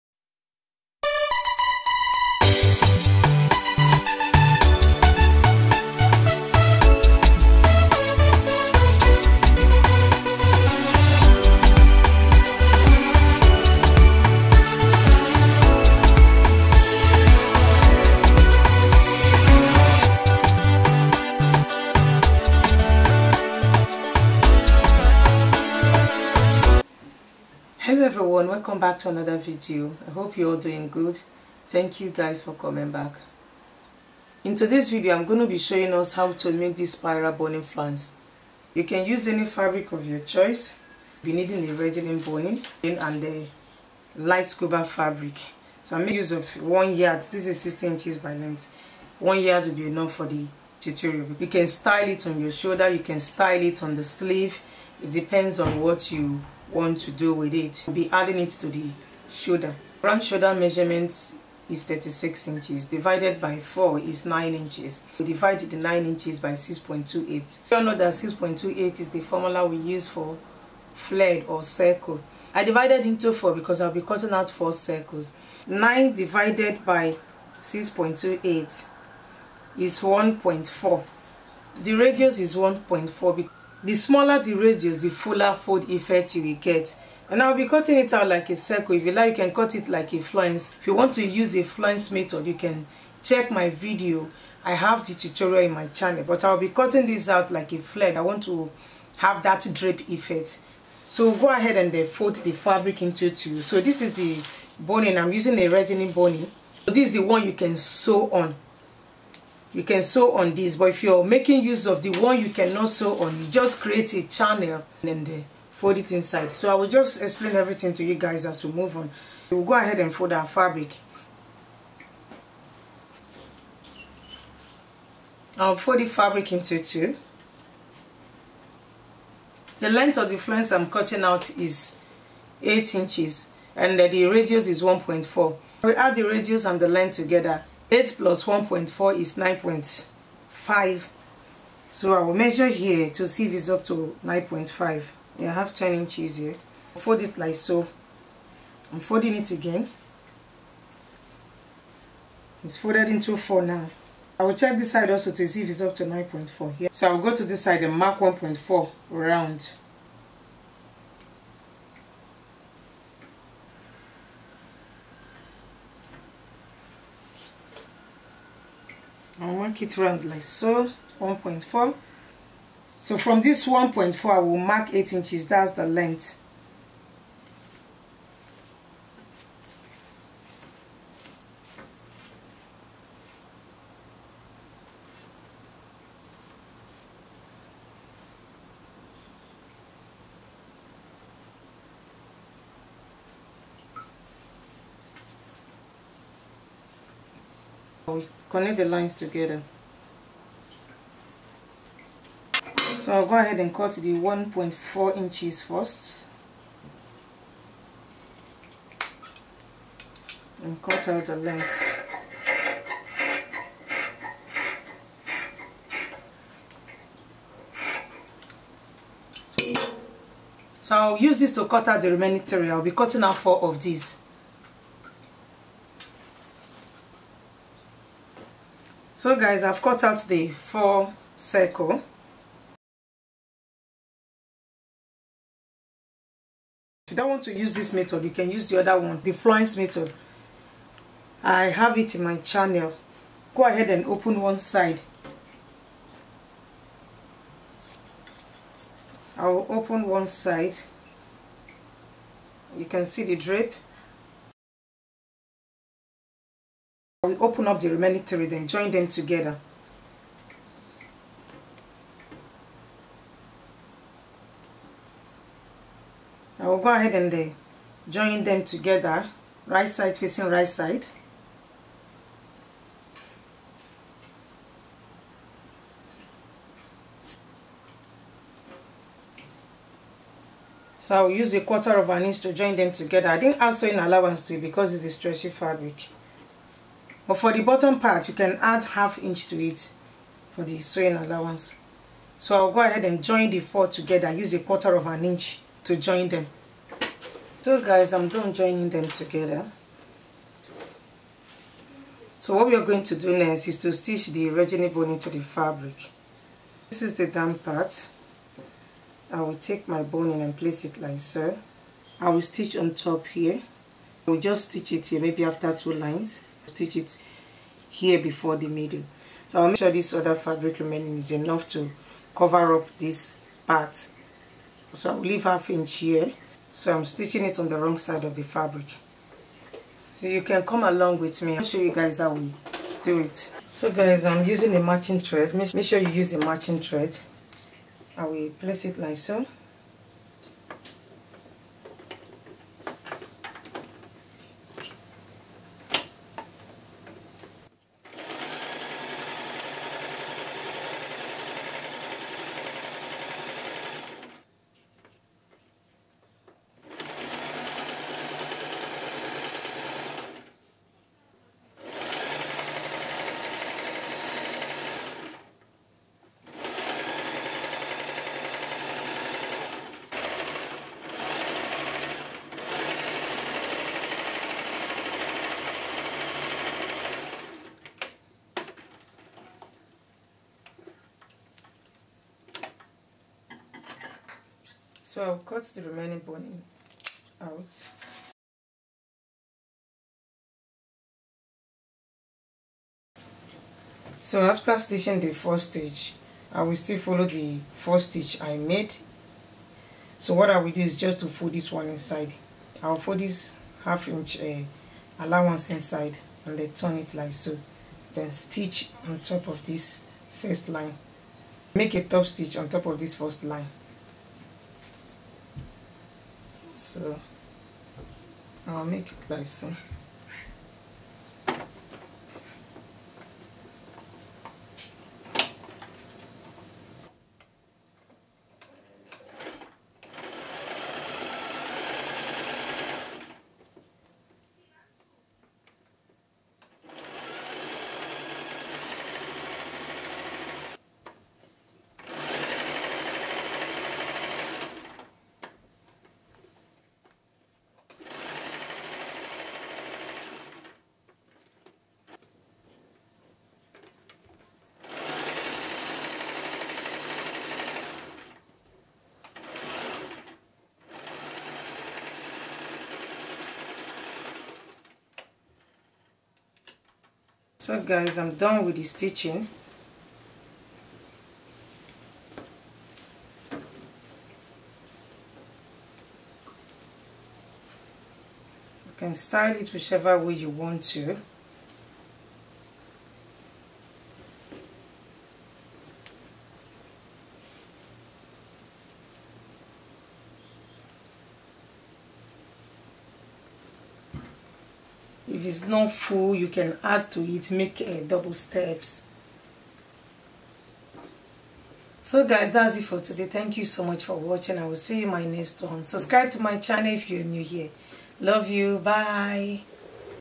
HOW TO MAKE A SPIRAL BONING FLOUNCE//CUTTING AND STITCHING/SPIRAL FLOUNCE
Items used
I used 1 yard of light scuba fabric
And rigilene  boning.